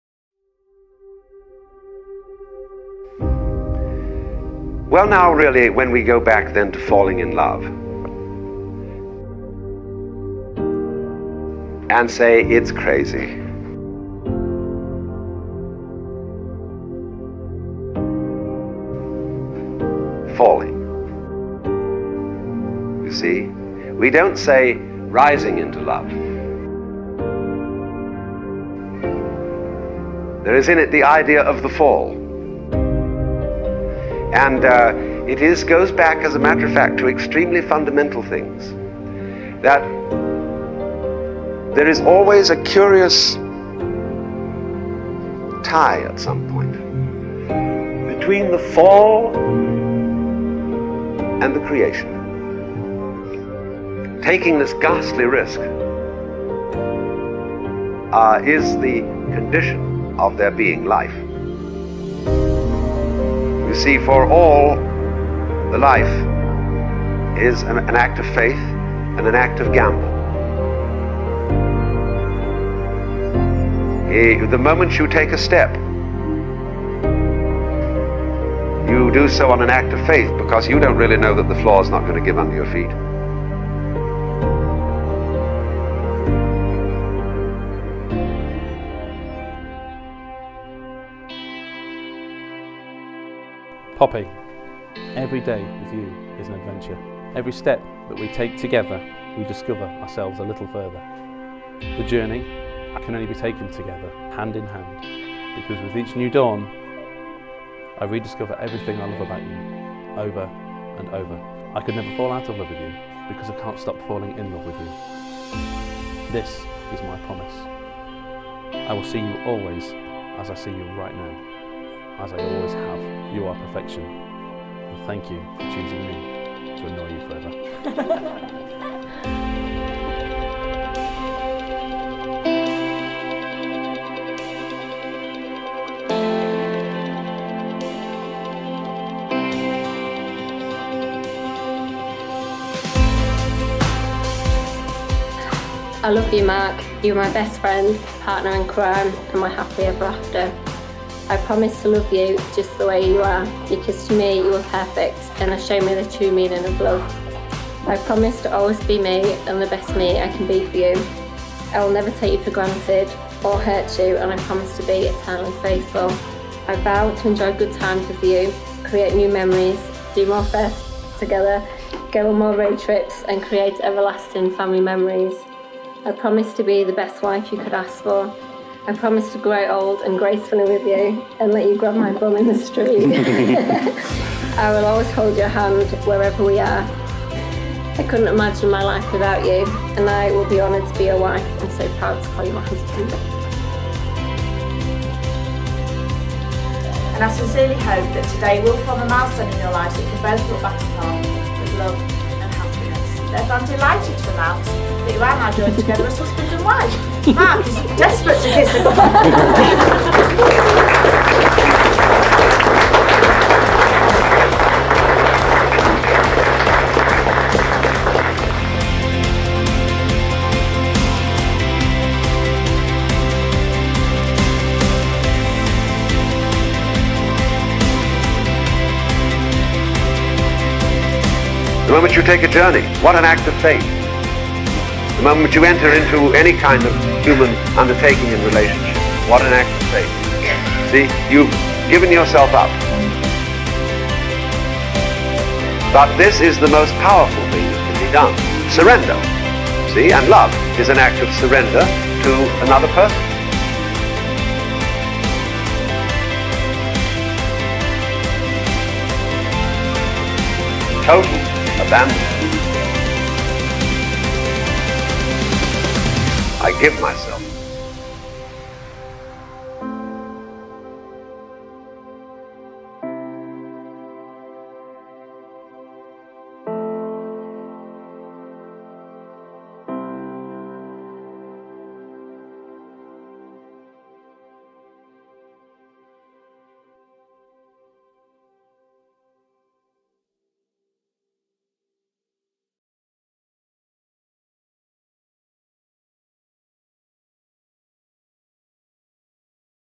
"I will never stop falling in love with you" Mark & Poppy Wedding Highlight Film
The most powerful wedding film you are likely to see.
Mark and Poppy wedding film by Daniel Marshall & Marea Fowler